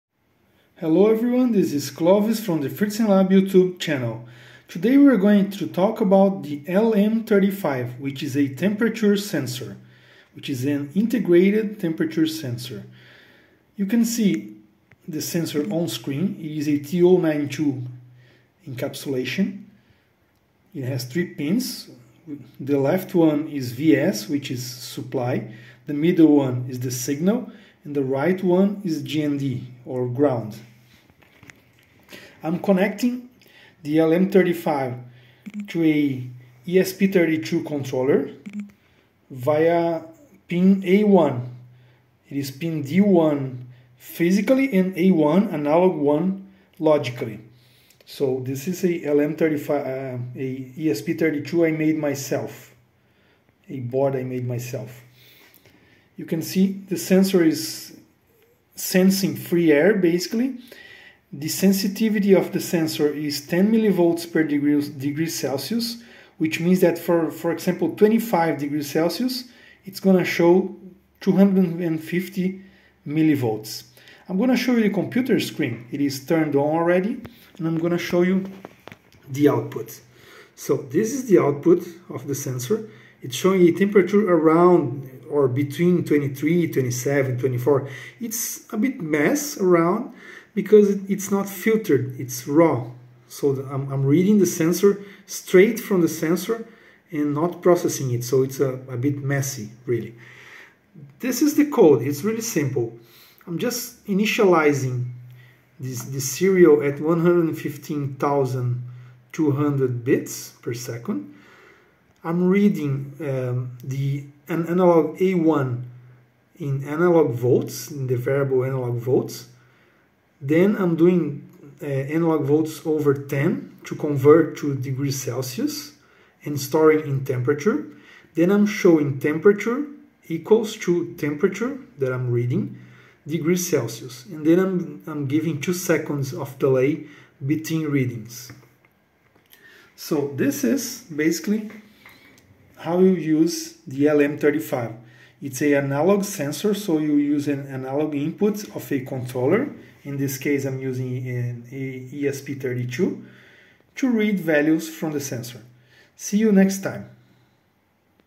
Temperature sensor with microcontroller: LM35 and ESP32
Learn how to interface a temperature sensor with ESP32: LM35.